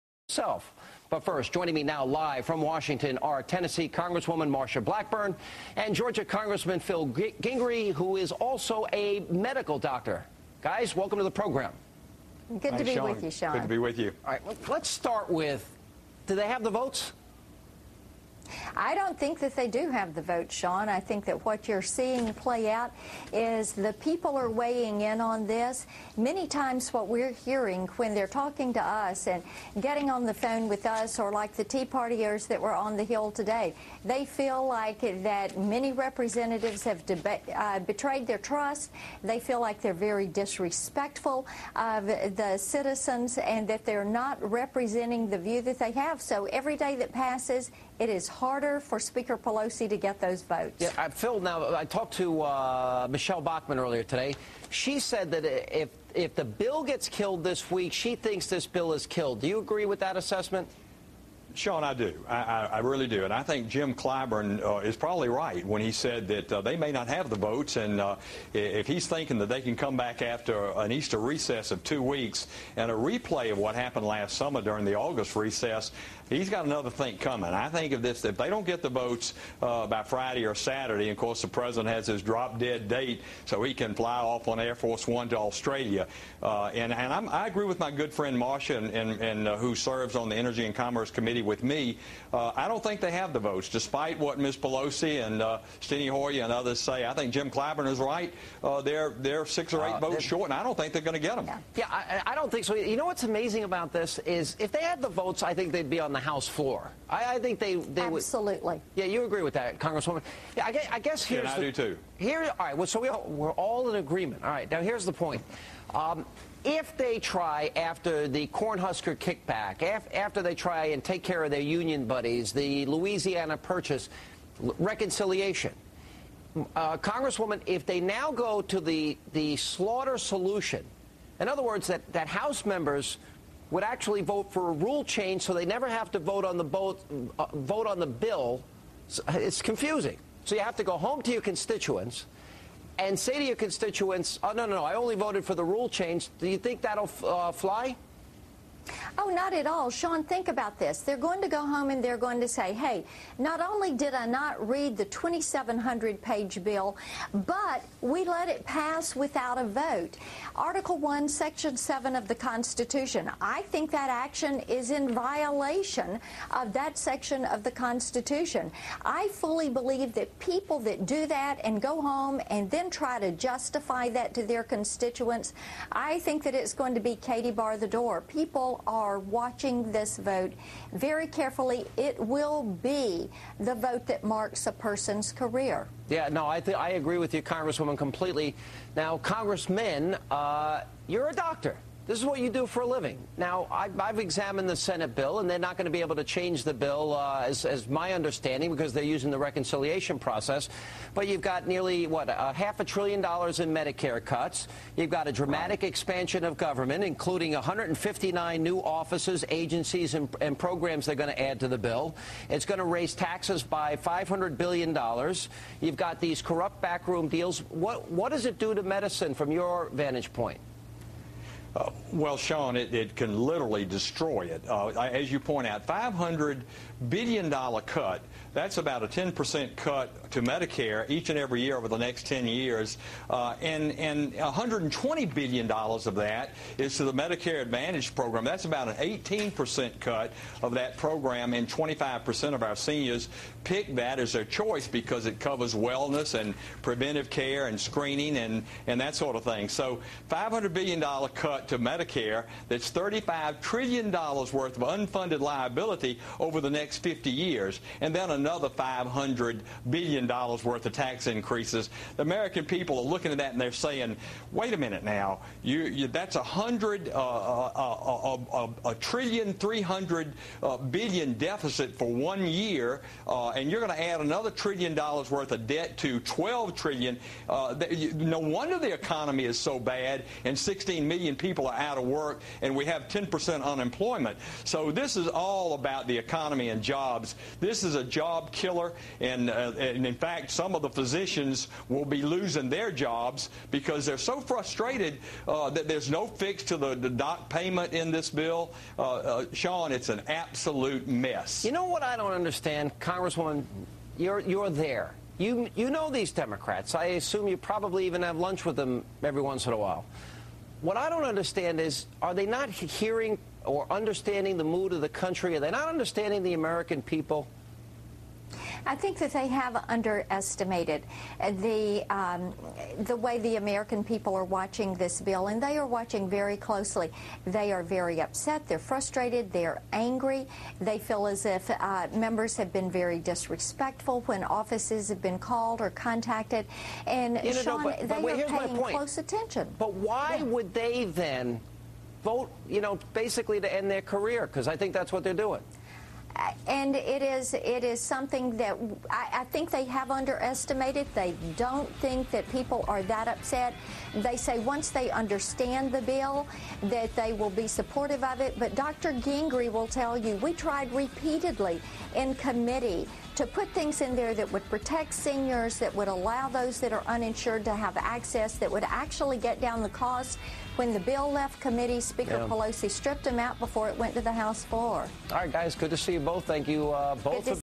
HEALTH CARE: Blackburn On The Vote Count
Congressmen Blackburn and Gingrey discusss member support for the health care bill.